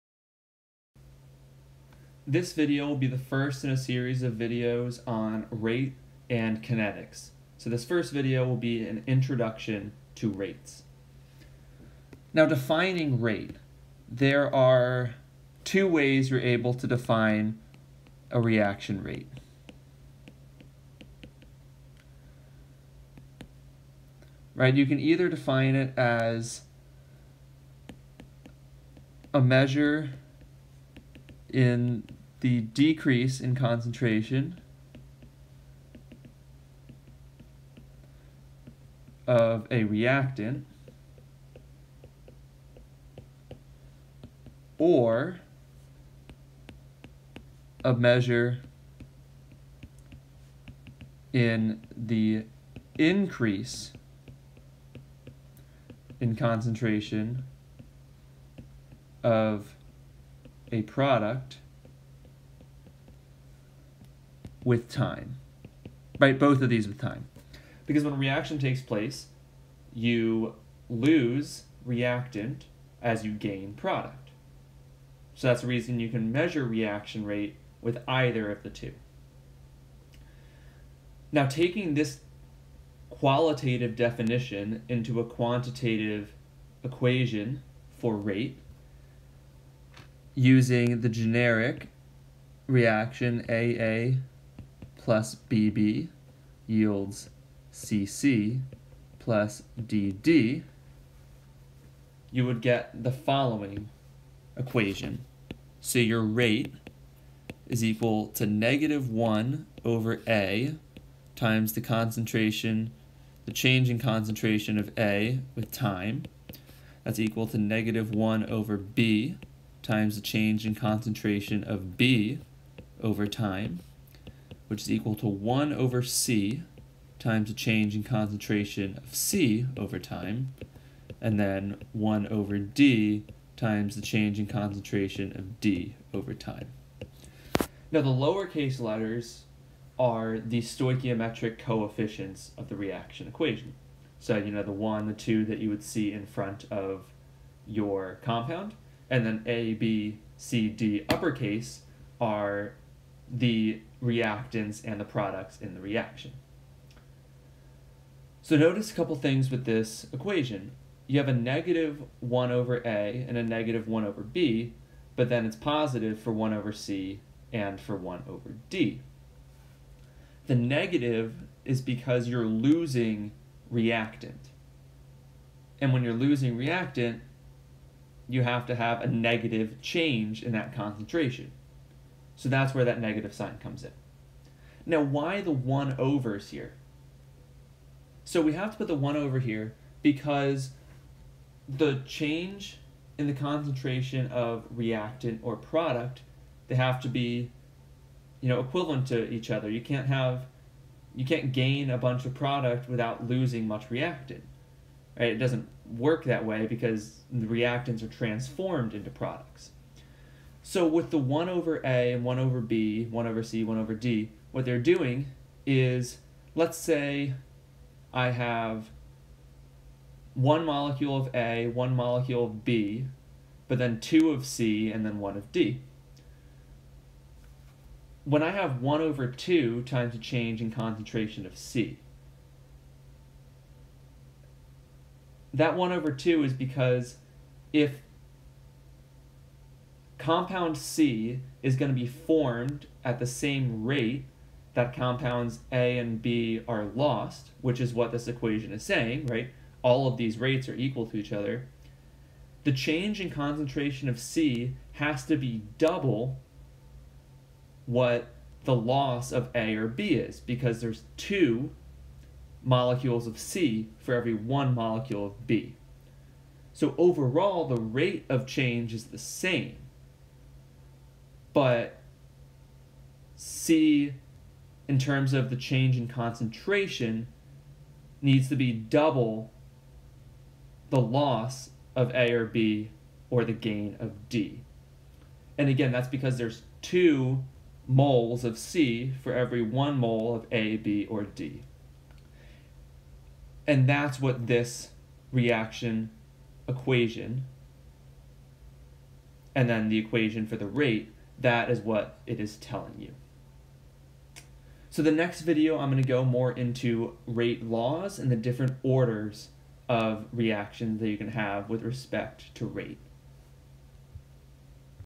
Introduction to Reaction Rates
The first in a series of videos on rate and rate laws, the video is a brief introduction to reaction rates.